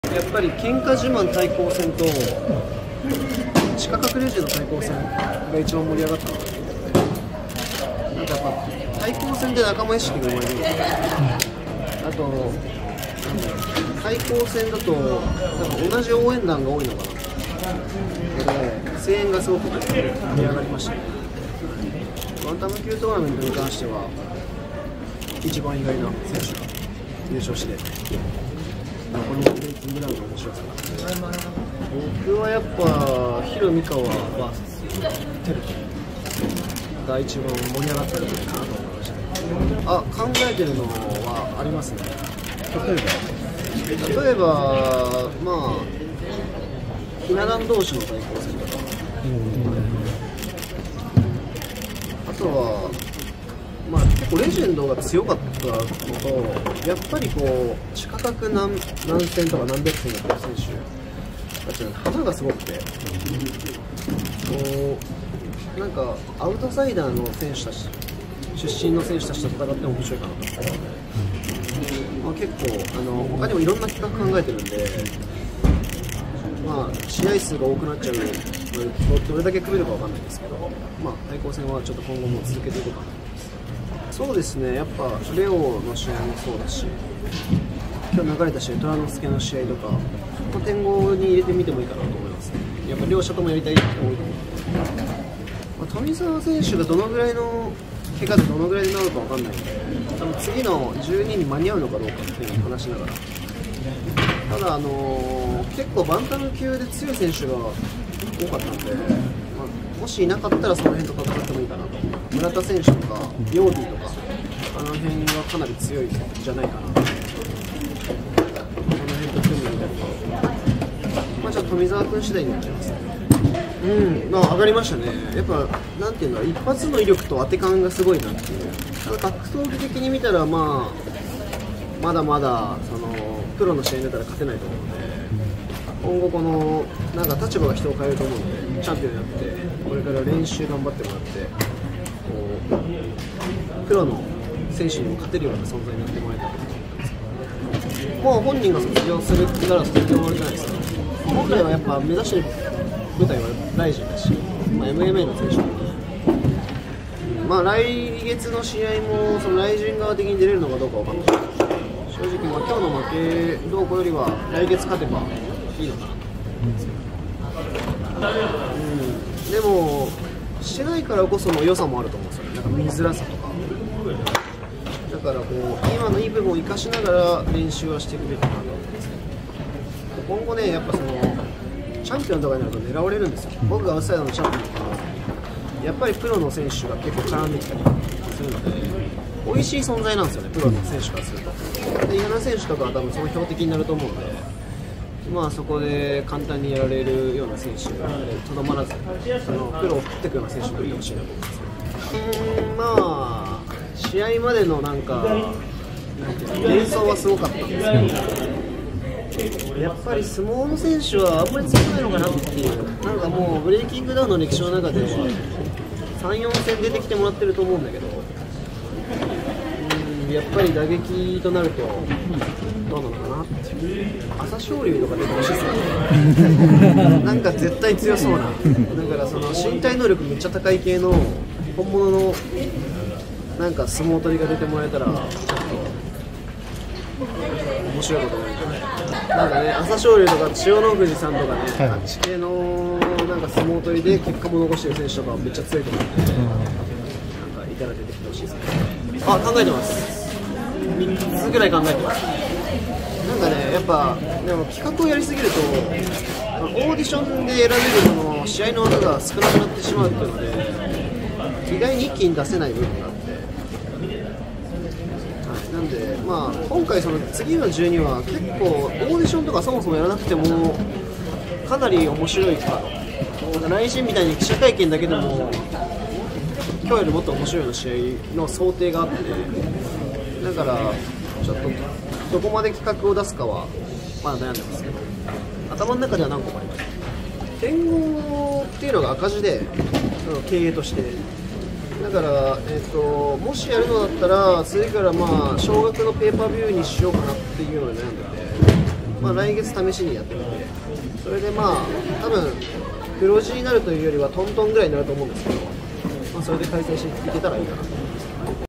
朝倉未来CEOがBreakingDown11を総括「一番盛り上がったのは…」元横綱に参戦呼びかけ
▼BreakingDownは朝倉未来チャンネルで
総合格闘家、朝倉未来（３１）が団体のＣＥＯを務め、１分間最強を決める格闘技大会「ＢｒｅａｋｉｎｇＤｏｗｎ１１（ブレイキングダウン１１）」が１８日、東京ドームシティ内プリズムホールで行われた。大阪と大宮による喧嘩自慢対抗戦や「ブレイキングダウンｖｓ地下格レジェンド」の対抗戦、バンタム級トーナメント決勝など２９試合が行われ、朝倉は大会を総括した。